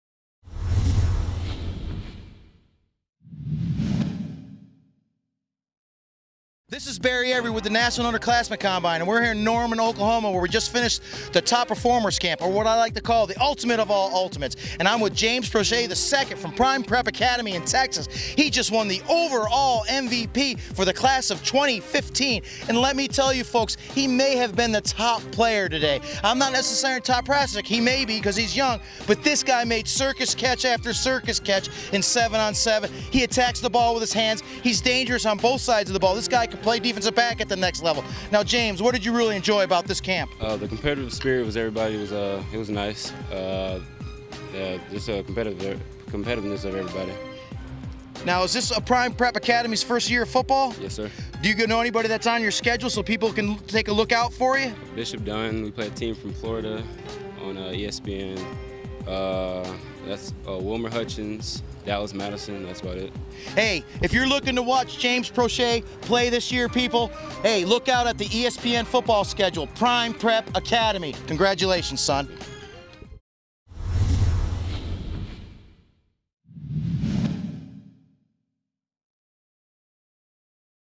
NUC - James Proche II - Top Prospect Elite 2012 - Class of 2015 Overall MVP
Watch our interview with James Proche II, the Class of 2015 Overall MVP from Prime Prep Academy, talk about his experience and performance at the 2012 Top Prospect Elite Camp by National Underclassmen.

Source: Freeplaymusic